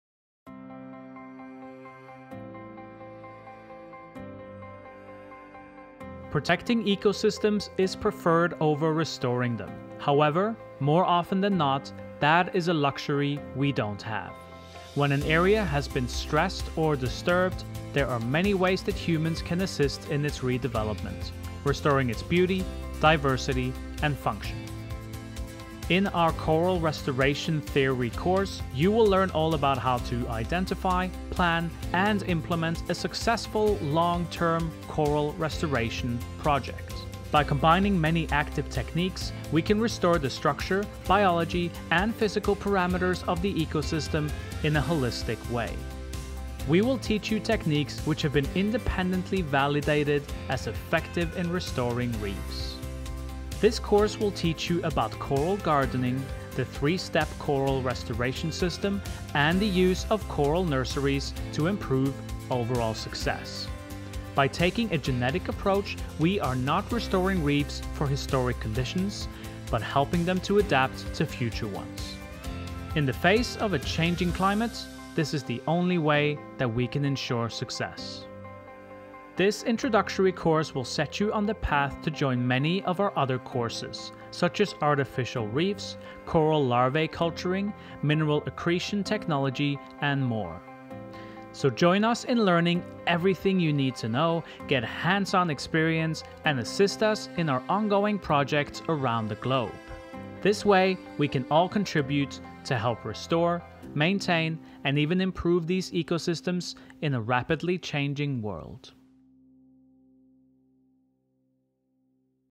Restoring Coral Reefs with Conservation Diver
In our coral restoration theory course you will learn all the basics about our genetically based, holistic method of reef restoration. Unlike many courses which focus on just the coral animals, our courses focus on the ecosystem as whole, based not on past conditions, but on future ones. As our planet changes due to the effects of climate change out thinking and our techniques must also change. Our program will give you the knowledge and skills that are on the vanguard of this industry.